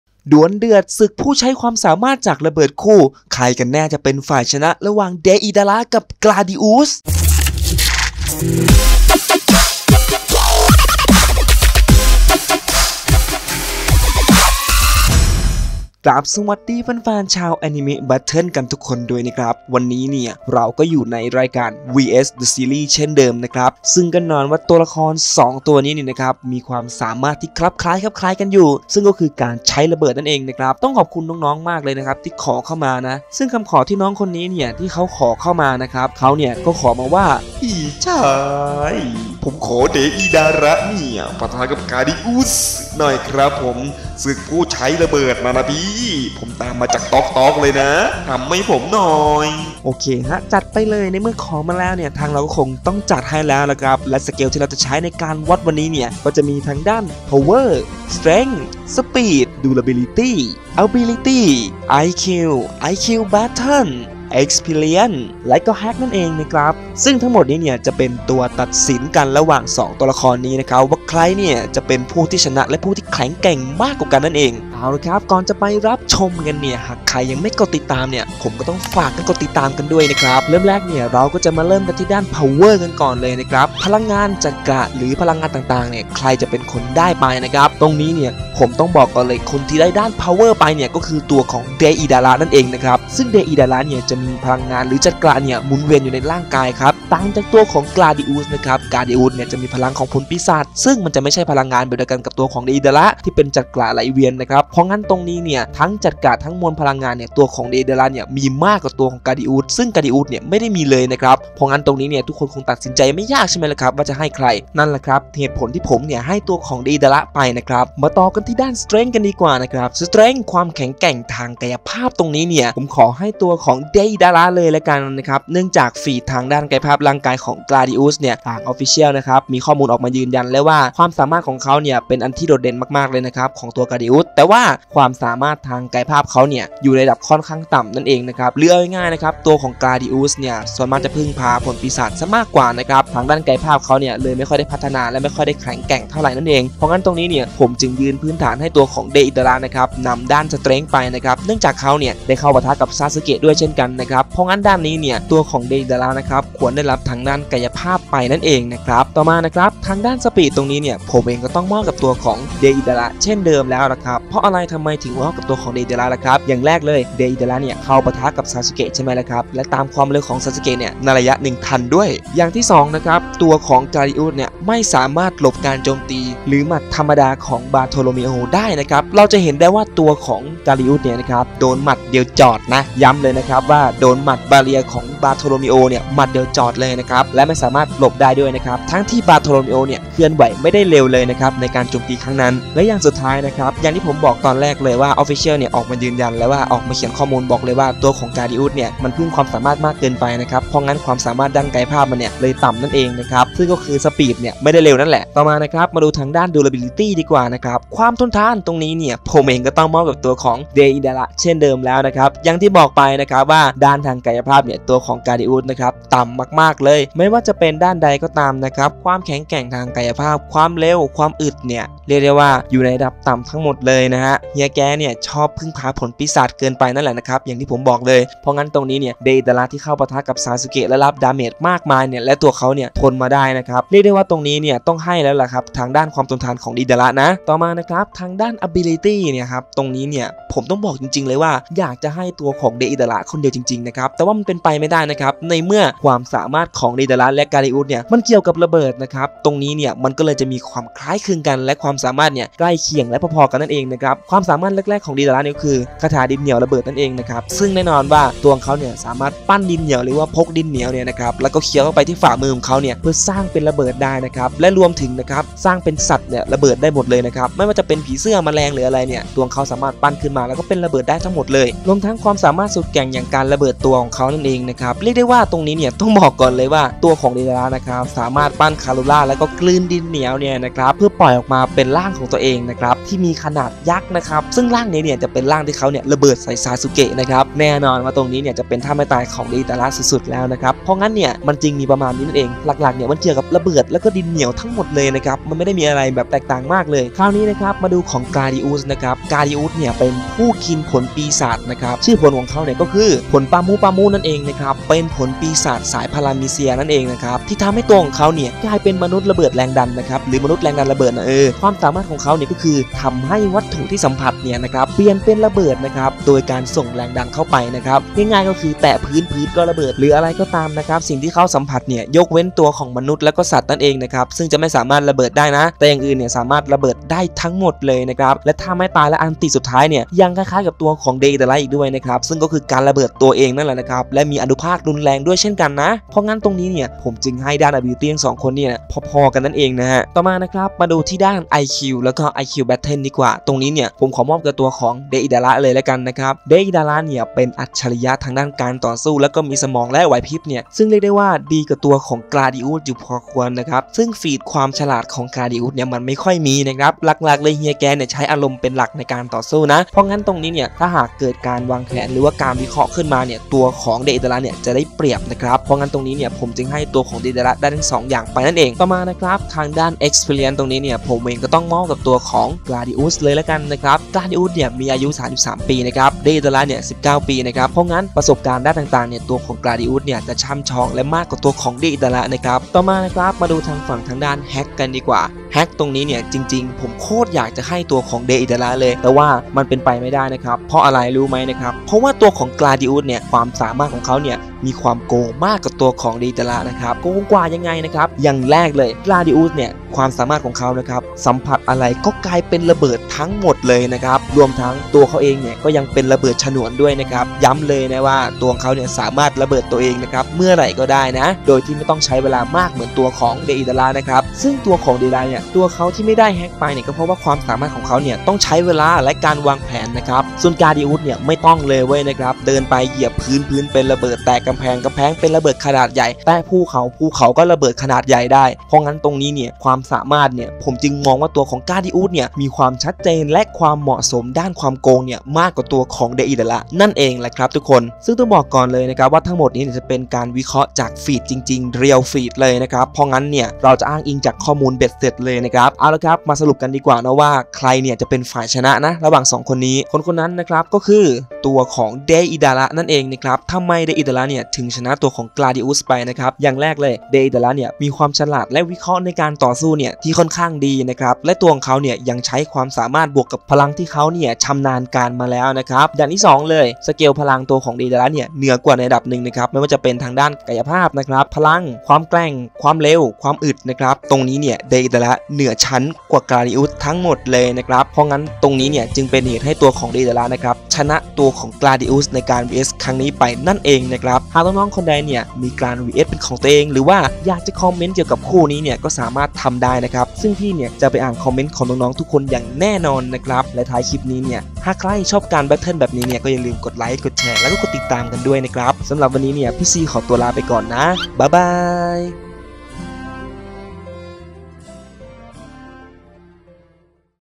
VS Battle - ดวลเดือดศึกผู้ใช้ระเบิด!!! เดอิดาระ ปะทะ กาดิอุส - {Anime Battle}
การ vs เบื้องต้นเป็นเพียงการวิเคราะห์จากคําพูดตัวละครโดยแท้จริงโดยจะไม่อ้างอิงจากเว็บ vs wiki battle
     ข้อมูลดังกล่าวนํามาเพื่อซึ่งการวิเคราะห์โดยตรงหากมีประเด็นไหนที่อยากเก็บมาพูดคุยสามารถกดติดตามช่อง Anime Battle ได้เลย
' การนํามา vs เป็นเพียงการคาดการและการวิเคราะห์ตัวละคร ตัวอนิเมะไม่เคยสู้กันจริง
เพราะฉนั้น ฟังและดูด้วยความบรรเทิงนะครับ